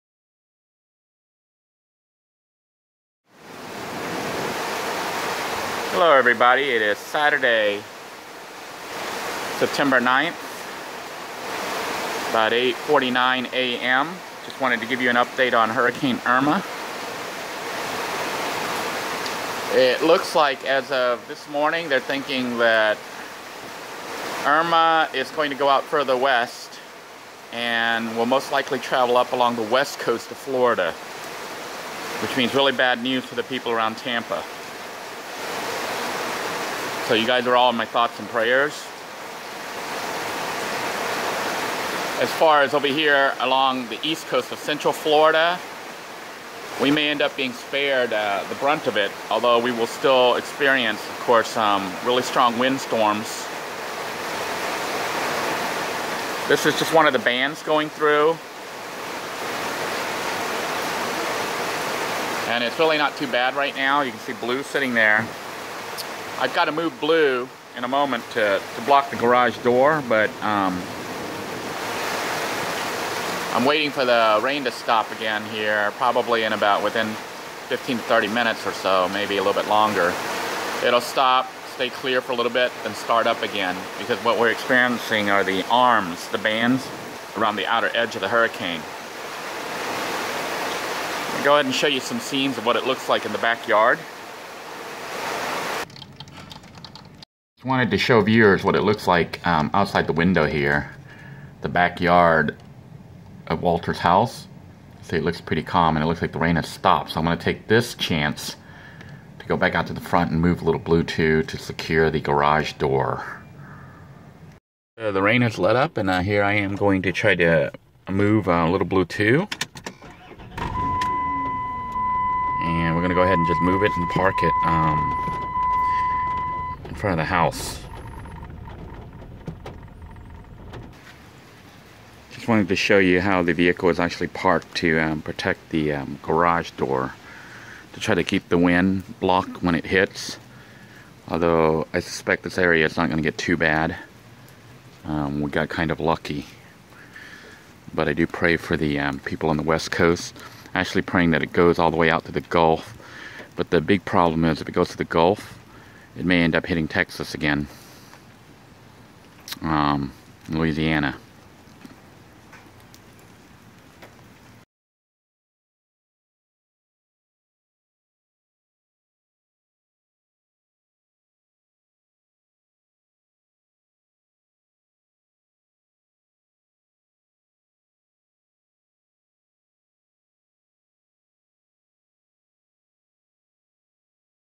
Final Preparations As Hurricane Irma Hits Melbourne (V867)
I finish up some last-minute prep for Hurricane Irma and then it is time to sit back and pray and stay up all night.  Power and internet went off at about 10 p.m. and stayed off till around 9:30 a.m. the next morning.  Not much damage was sustained at Walter's House but the neighbor next door...well...that is another matter...